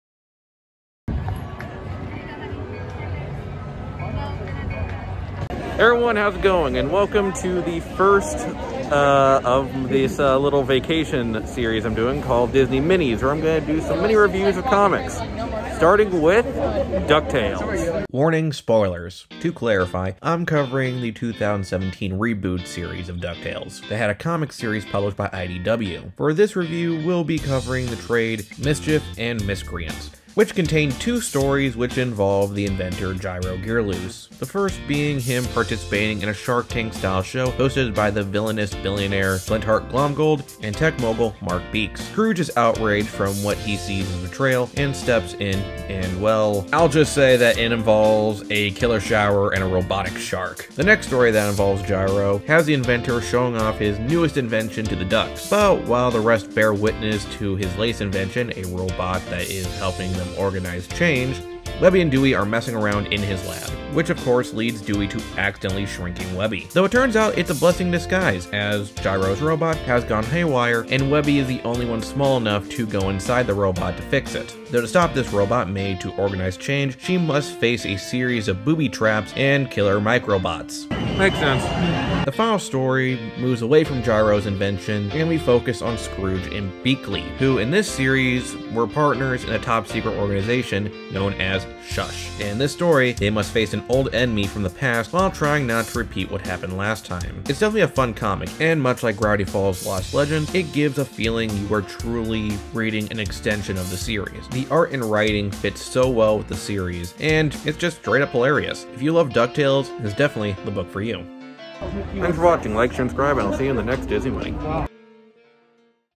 Ducktales: Mischief and Miscreants (Mini-Comic Review)
At Disneyland, but that doesn't mean I ain't gonna talk about some comics. We fallow the theme of Disney as we start "The Disney Minis" with Ducktales 2017's adventures in IDW Comics